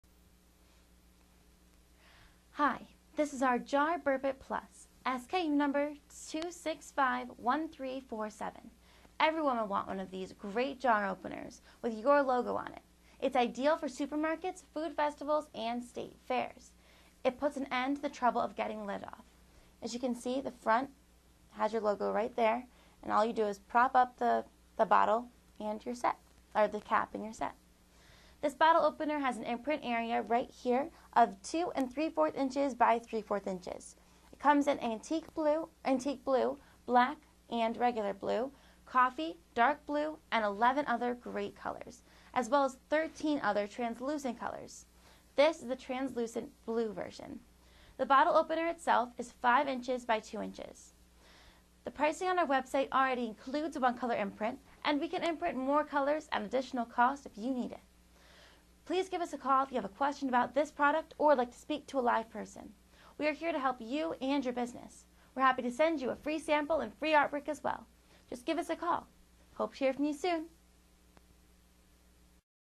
Jar BURP-IT PLUS
Your customers will line up to get one of these with your name on it! This jar opener puts an end to trouble getting the lid off. Also can get off the tops of some water bottles. Ideal for supermarkets, food festivals and state fairs. Dishwasher safe. Made in the USA. Please call for more details. SKU: 265-1347